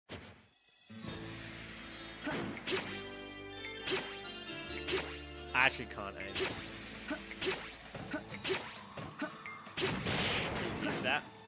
Poppt1 Can't Aim
The so-called sniper in the building can't hit a target about 10 feet away; either he's become out of his element, or he's too used to wii sports bowling. Nice job Jordan, quality content for the stream.

➤ Wanna help us with stream highlights? Hit us an email, clips, thumbnails, feel free to help in anyway you'd like!